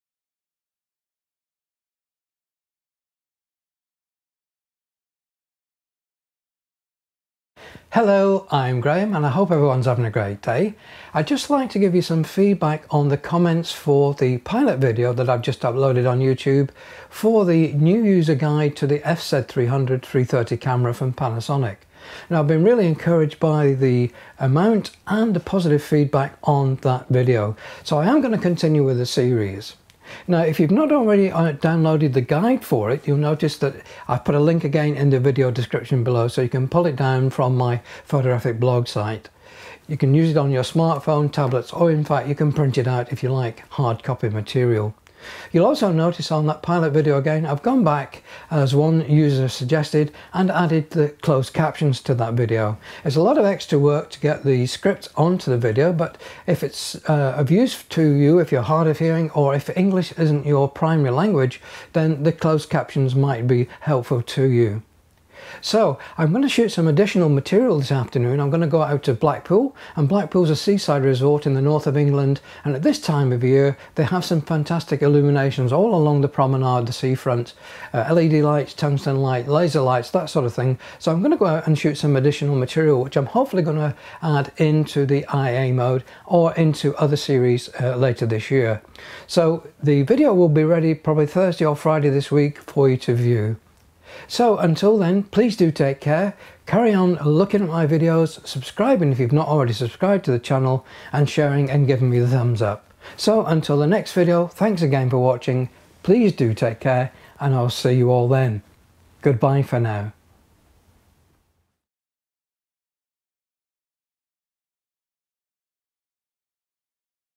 FZ300/330 Beginners Guide - pilot video - your feedback
Feedback from the pilot episode for the new series for beginners using the FZ300/330 camera.
Thank you to everyone who commented, git it the thumbs up and made suggestions for the future episode.

Link for the pdf file